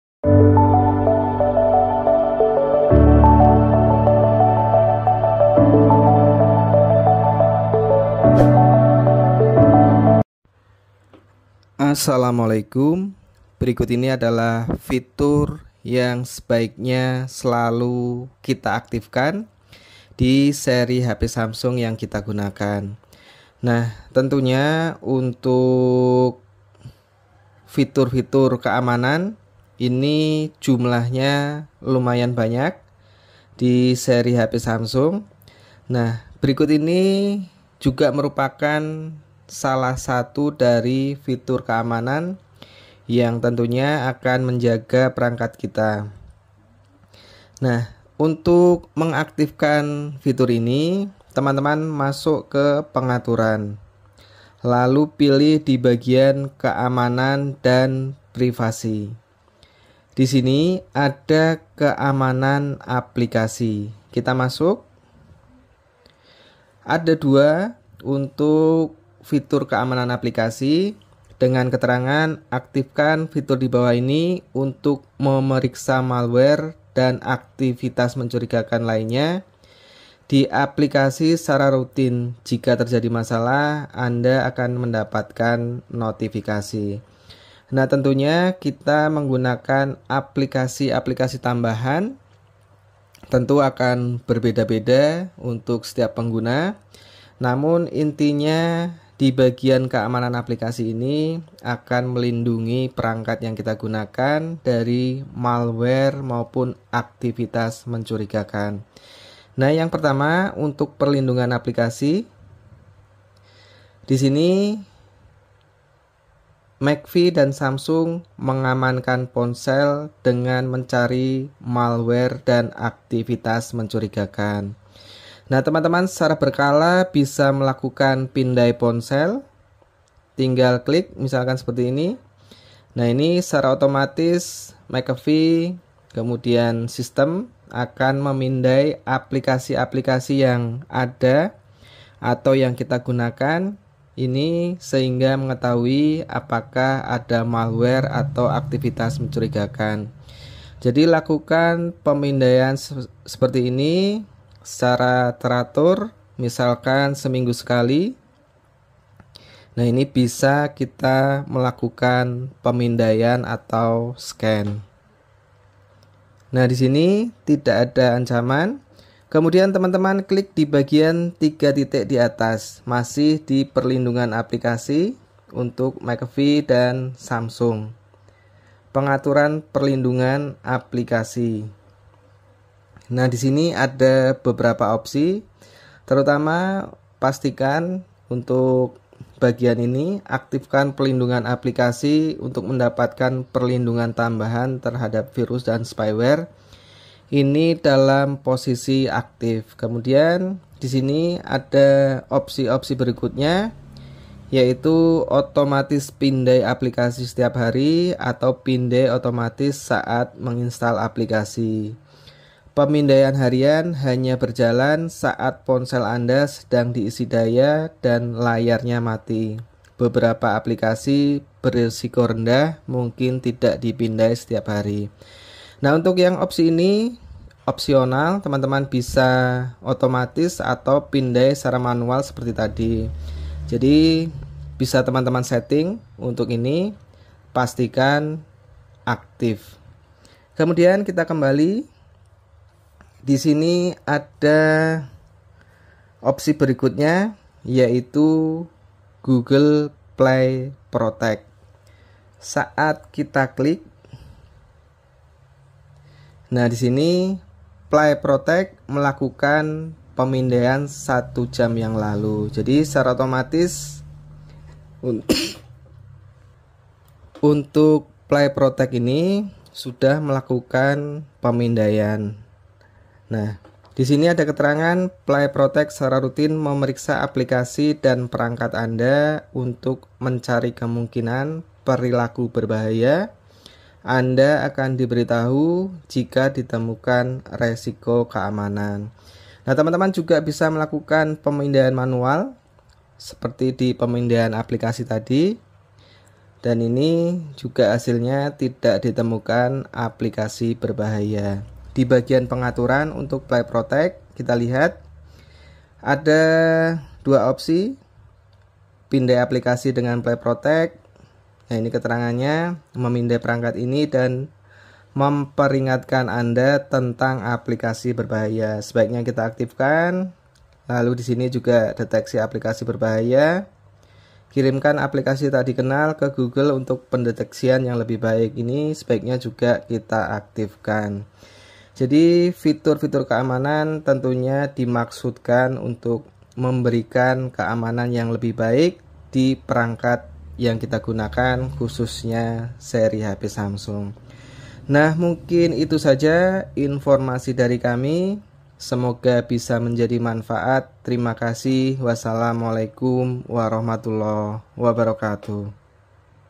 Fitur Keamanan Yang Sebaiknya Selalu Kita Aktifkan Di HP Samsung | Penting Dan Bermanfaat
Fitur Keamanan Penting Di HP Samsung Yang Sebaiknya Slalu Kita Aktifkan Dan Gunakan.